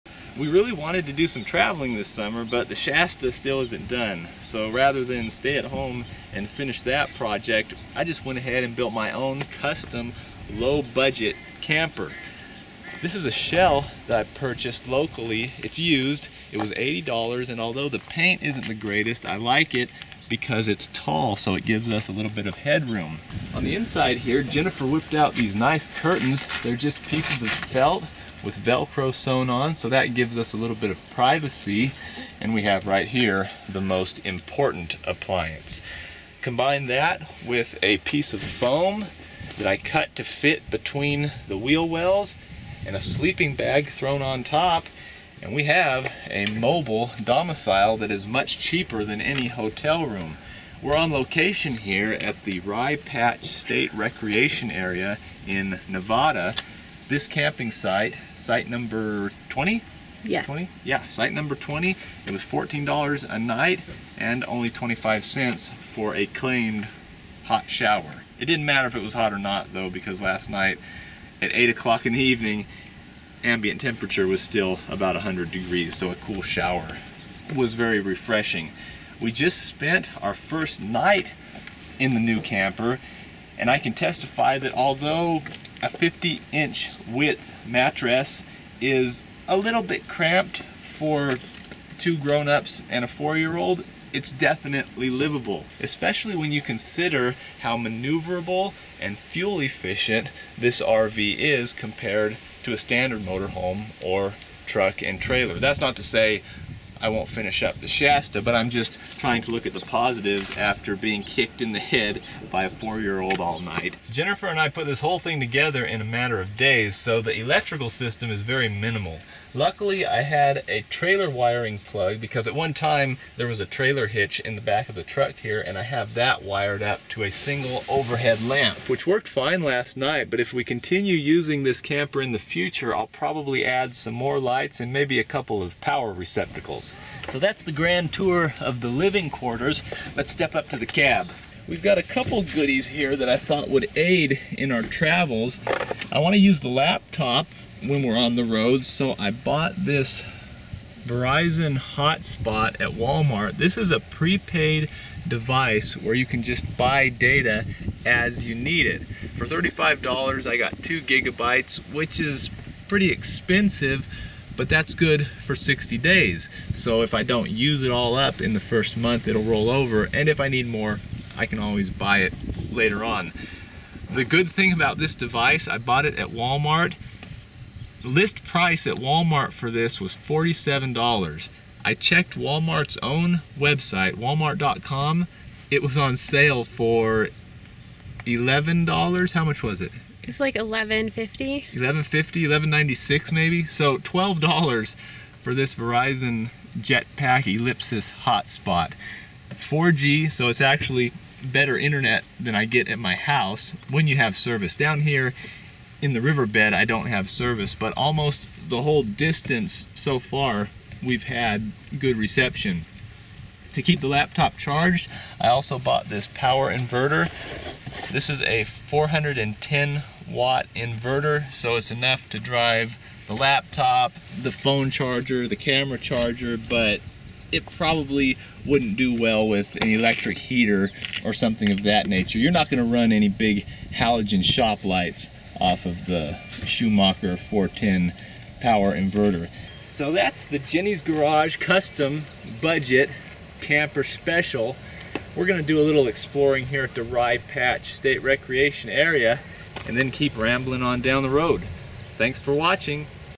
Truck Bed Camper!
My low budget truck bed RV for traveling.
Items:
Camper shell / Topper
4" foam mattress
Large sleeping bag
Thetford Porta Potti (round style)
Verizon Ellipsis Jetpack 2 Hotspot
Schumacher X141B 410W inverter